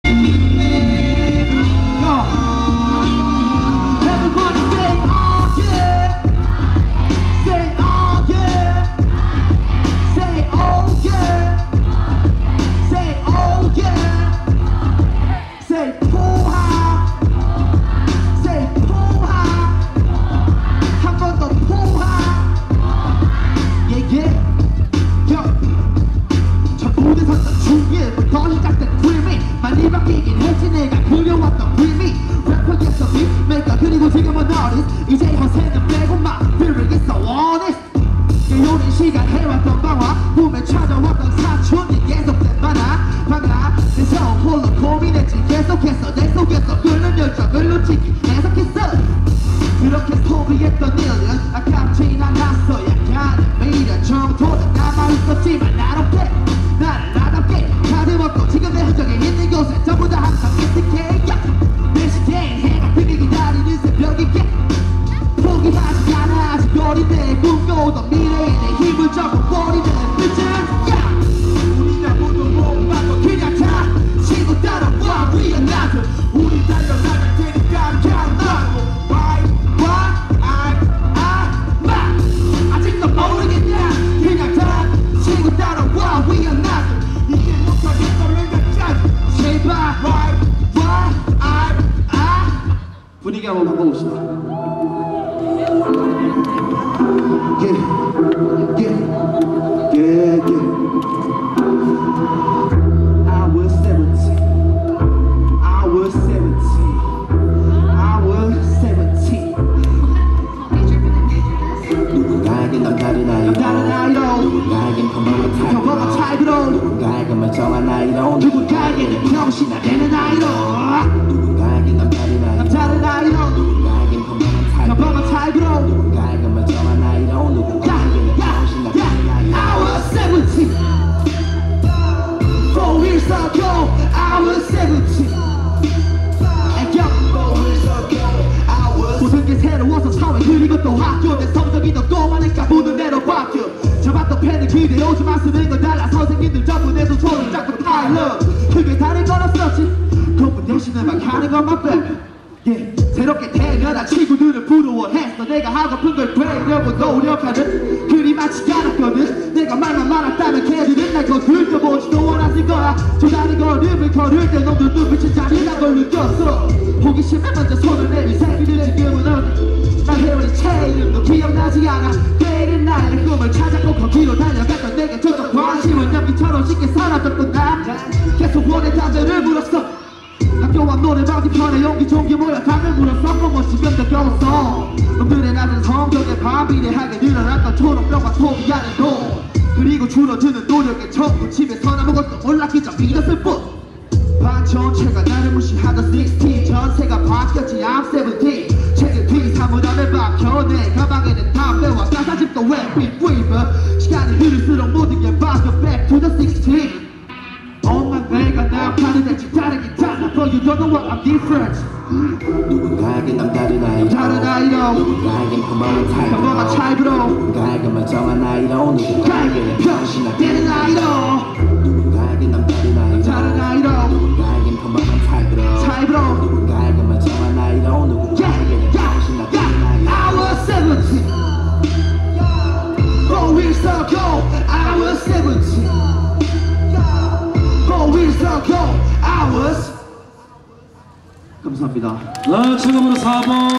(참가상 ) 포항불빛 힙합 콘테스트 -참가4번:Asi K - 김병혁
2017 포항 국제불꽃축제 , Pohang International Fireworks Festival